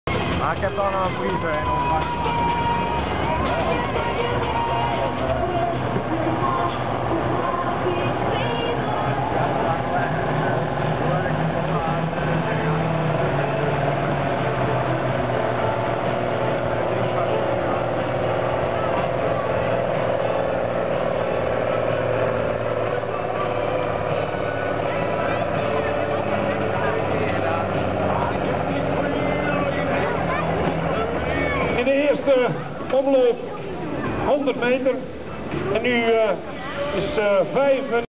Jeroen Peppelenbos met de Fendt 612 in Kamperveen
Trekkertrek Kamperveen 2011. Jeroen Peppelenbos op de bok van zijn Fendt 612 voor de Balance of Power sleepwagen.